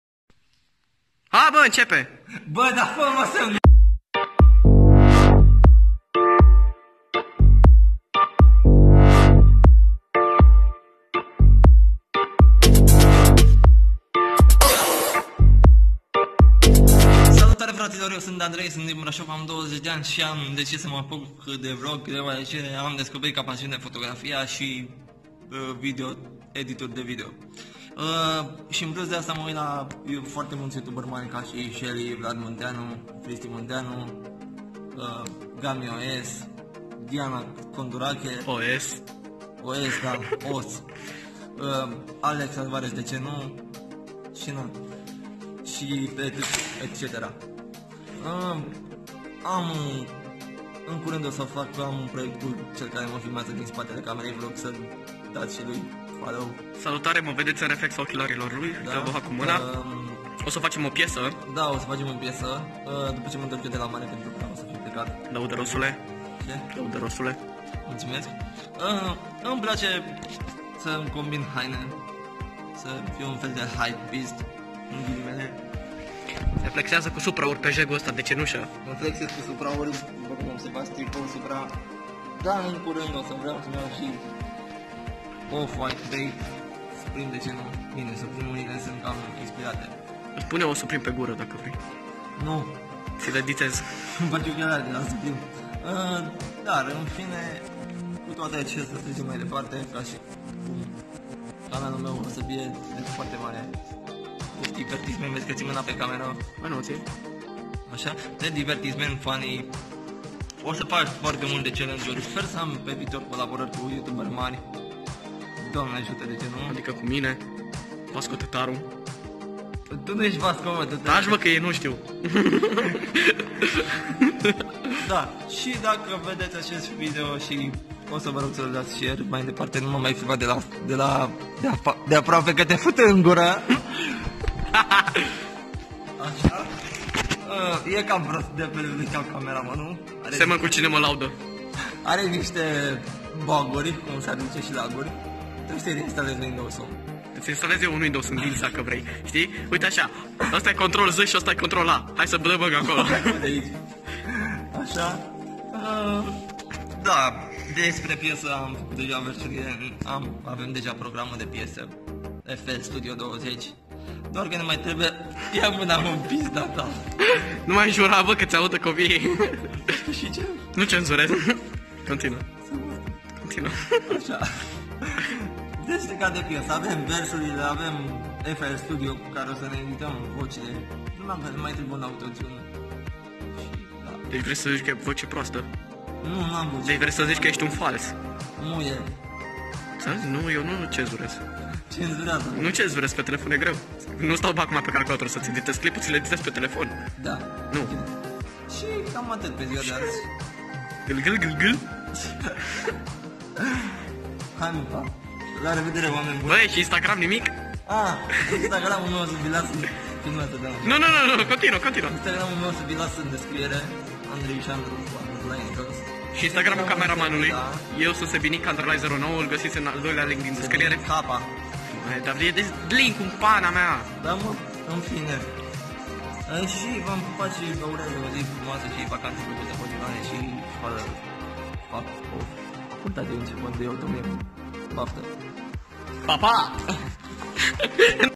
Primul episod | Andrei Sandru
Salut, asta e primul vlog al meu. Scuze de emoțiile de pe vlog dar era prima mea filmare de genu

Instagram-ul meu : @andreisandru_
Instagram-ul cameramanului : @sebinik_09